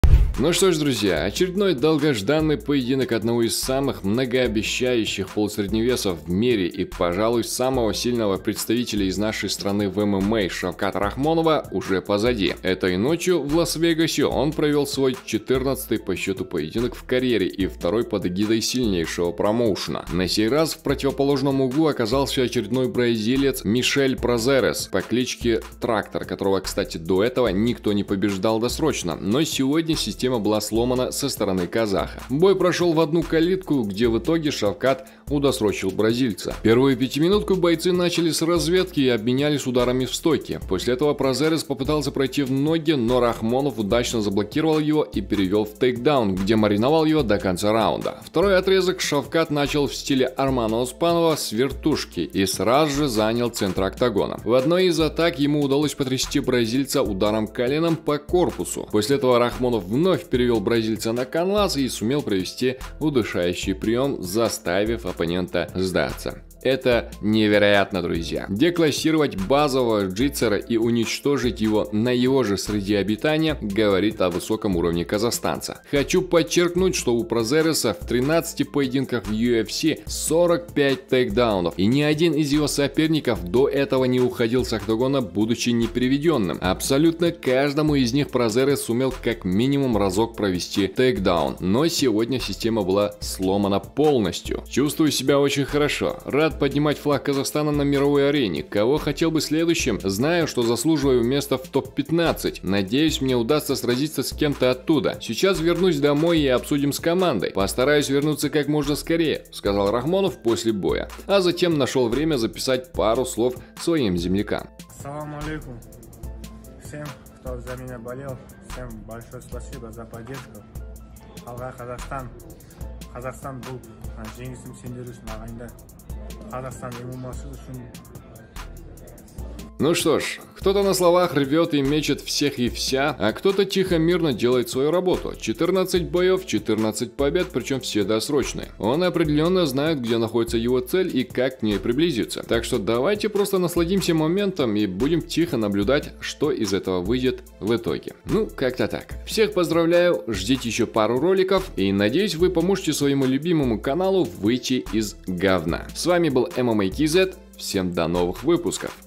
Шавкат Рахмонов - Мишель Празерес. КАЗАХ СДЕЛАЛ ТО, ЧЕГО НИКТО НЕ СМОГ! UFC Vegas30
Казахстанский боец Шавкат Рахмонов (13-0) сразился с бразильцем Мишелом Празерисом (26-3) на турнире по смешанным единоборствам UFC Fight Night 190 в Лас-Вегасе, сообщает Sports.kz.
Шавкат Рахмонов удосрочил бразильца с 26 победами в своем втором бою в UFC.
Их поединок в полусредней весовой категории был рассчитан на три раунда и закончился победой Рахмонова удушающим приемом во втором раунде.
Этот канал является членом медиа-сообщества Yellow Bridge. Все вопросы относительно рекламы адресовать в группу нашего сообщества «В Контакте»